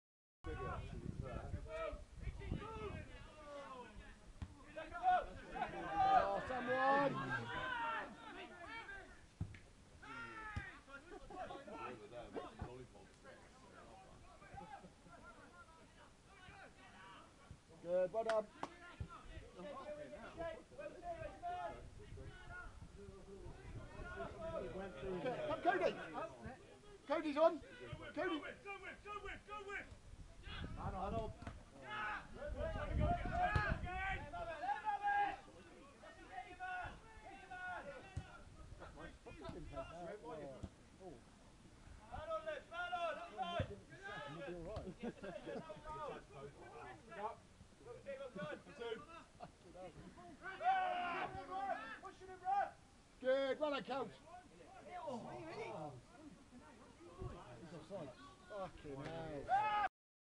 ECPL Premier League Game St Stephen 4 v 3 St Cleer Played today 10/10/20.
Some nice football and then Courtney Rowe lets a chance for a shot go bye.